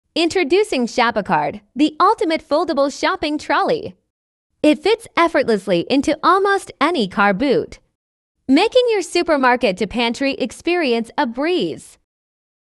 Shoppacart: Shopping Trolley for Any Car Boot
Introducing Shoppacart: a game-changing, foldable shopping trolley! Load and unload effortlessly from your car boot, enjoy a seamless supermarket to pantry experience. Ditch plastic bags, avoid shared trolley germs, and say goodbye to car park struggles.